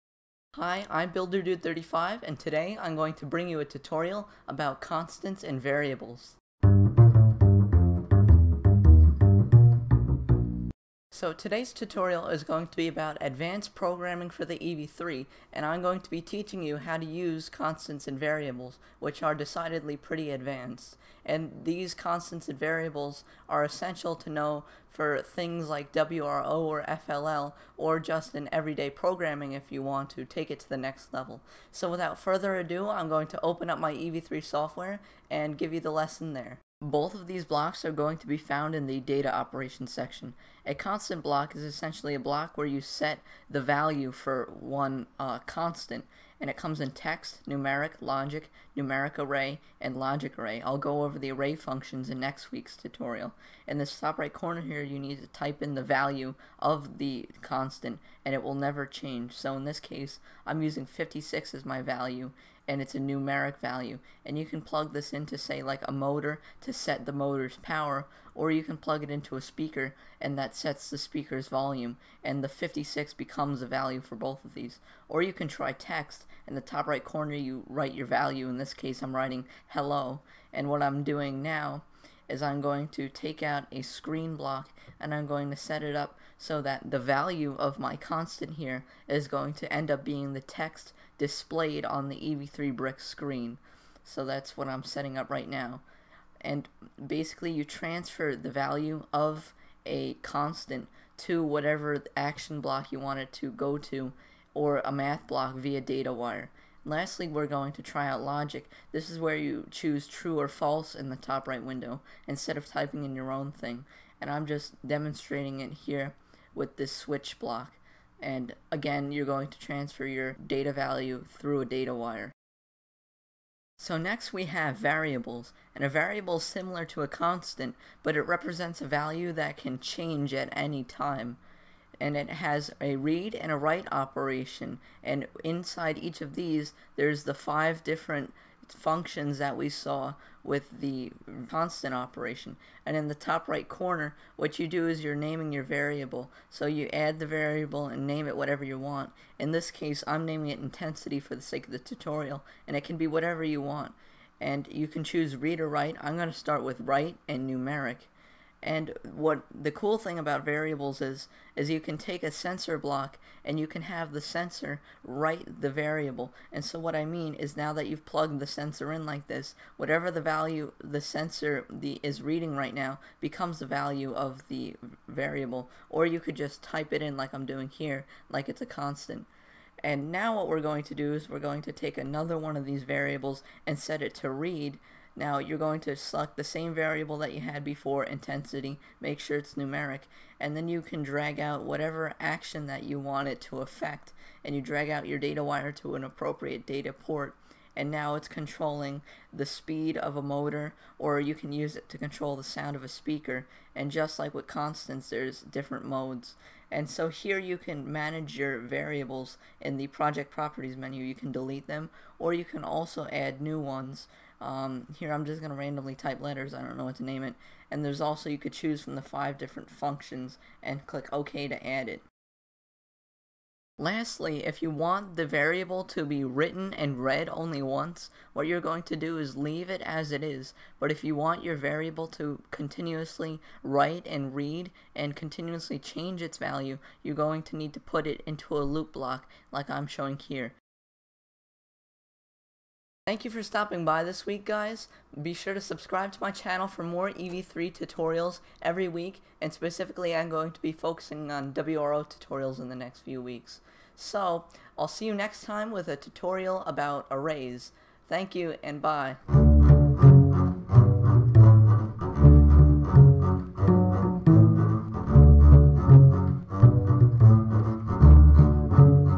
How to Program Constants and Variables (EV3-G)
In this EV3 programming tutorial, I will be explaining how to use constants and variables to program your EV3 robot and I will provide some examples for these advanced programming blocks. These blocks are important to learn in master for WRO, FLL or if you want to take your EV3 programming to the next level!